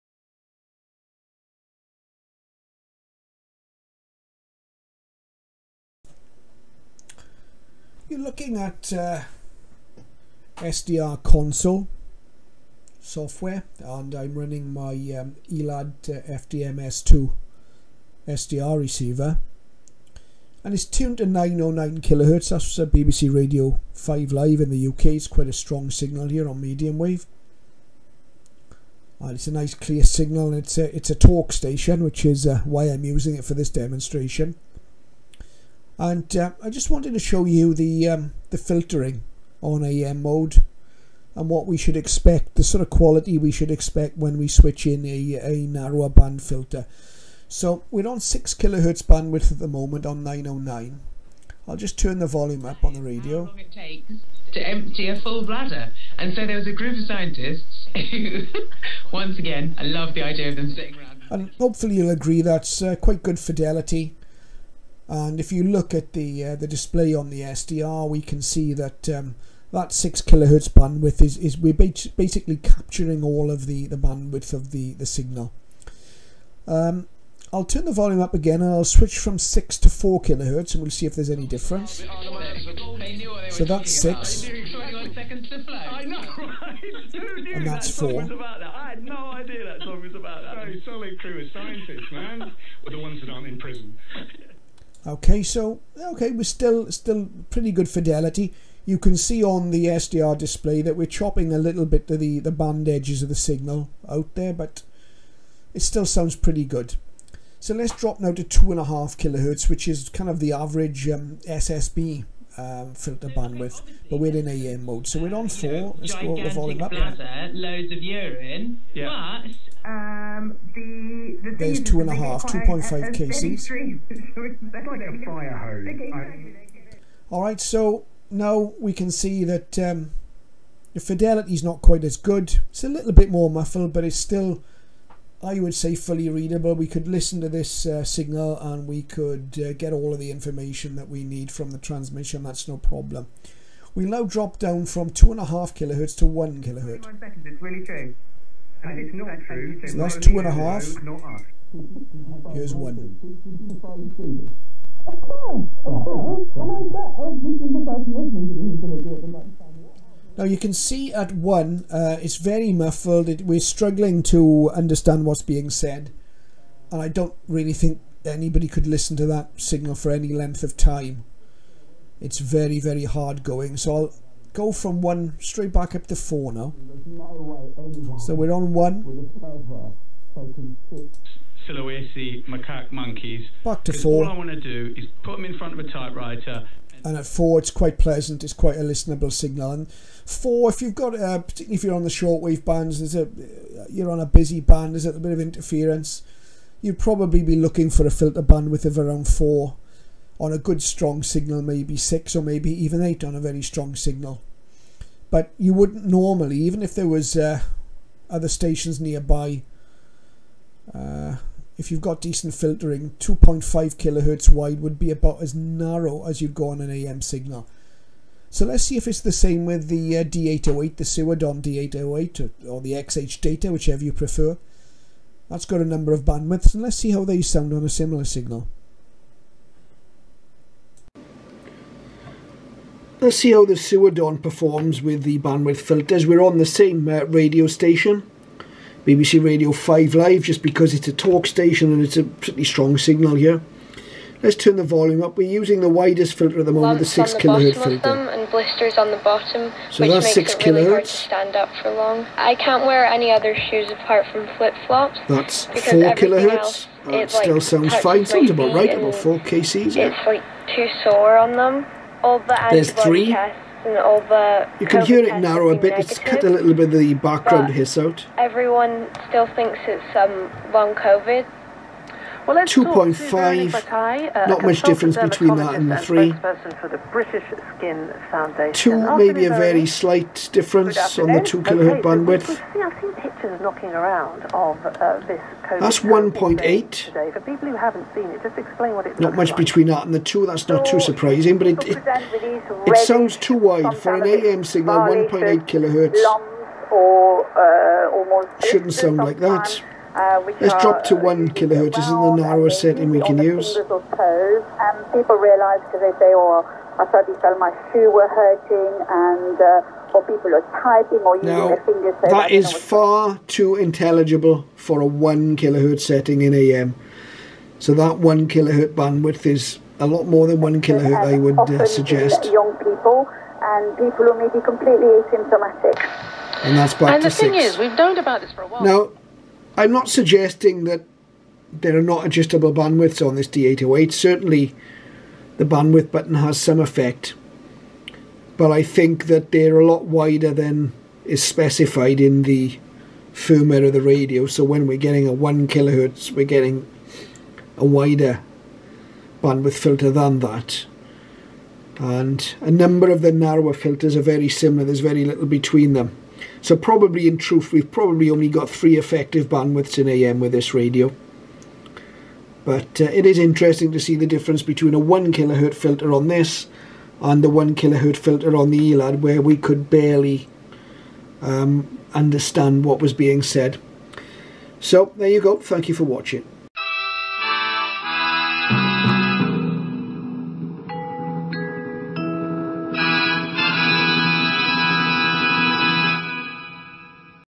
SIhuadon/XHDATA D808 bandwidths
The D808 appears to have a multitude of filters. But all is not what It seems. I demonstrate the filters on my Elad FDM SDR receiver and compare the filtering to the D808.